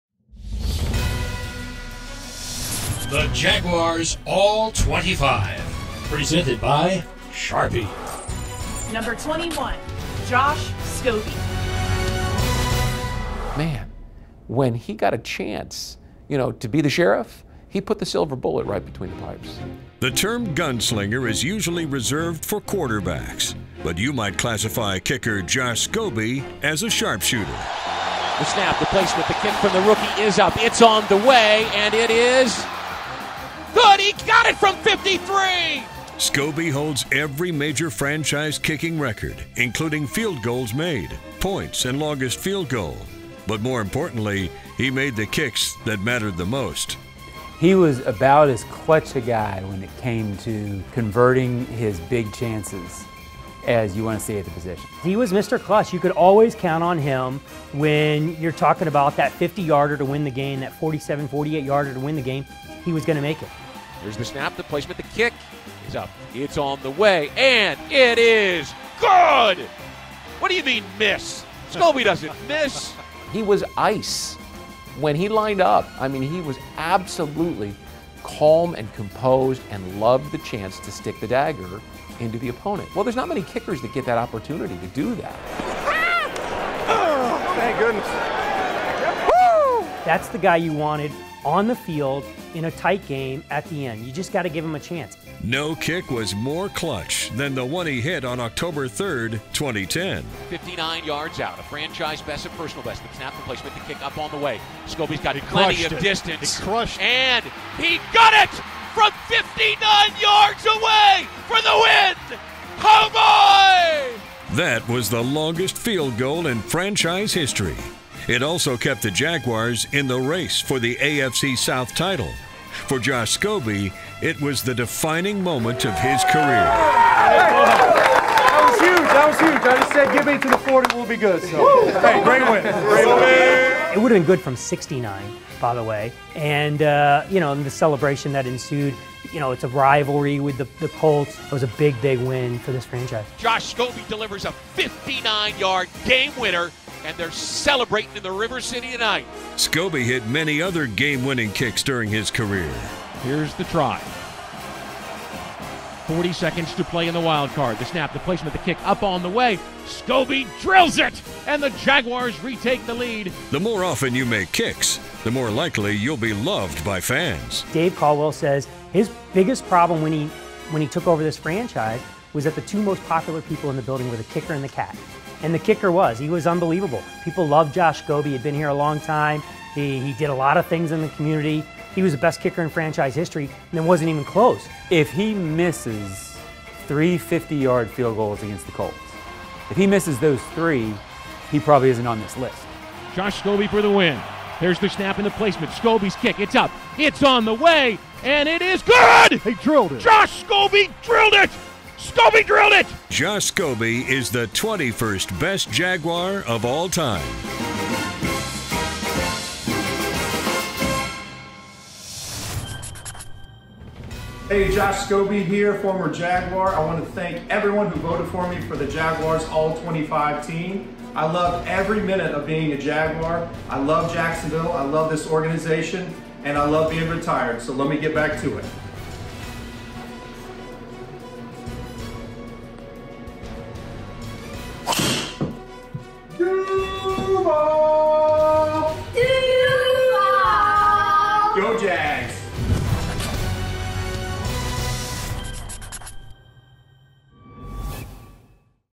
Jaguars All-25: #21 Josh Scobee
Josh Scobee is No. 21 on the Jaguars All-25, a collection of the top 25 non-active players in franchise history, as voted on by members of the local media.